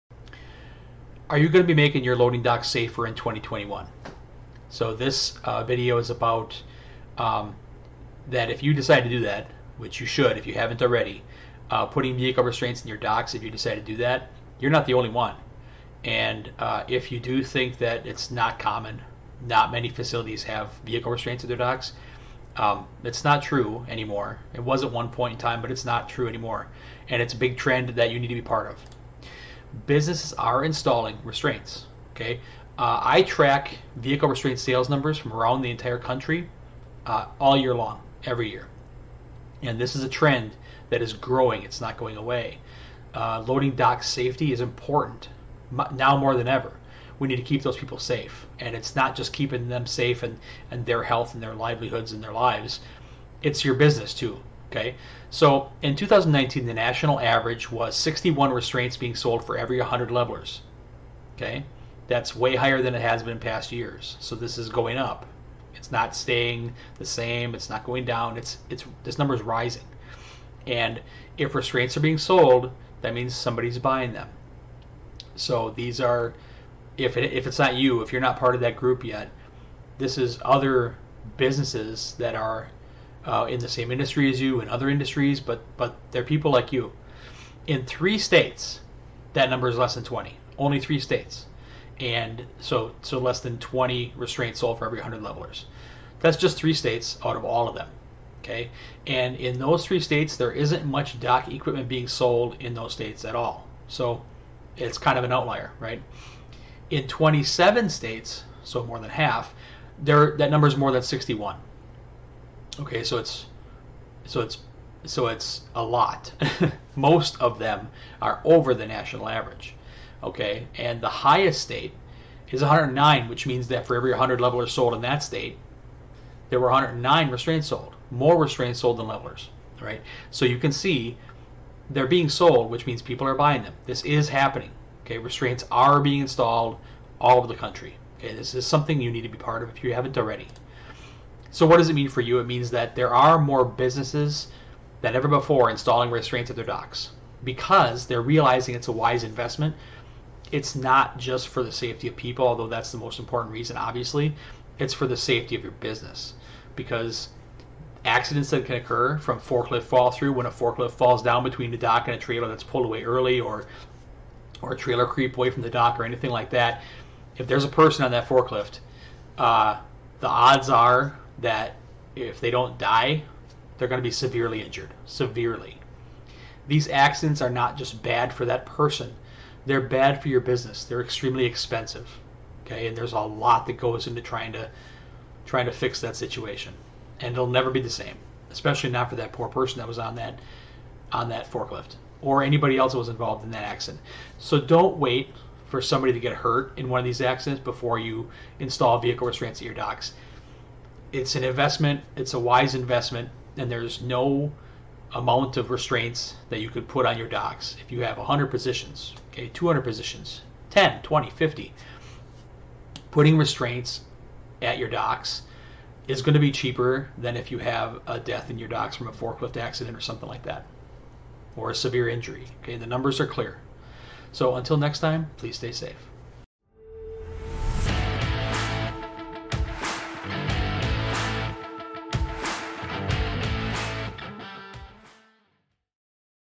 Will Your Loading Docks Be Safer in 2021?
Jeremy Artz goes in-depth on how vehicle restraints improve safety at the loading dock.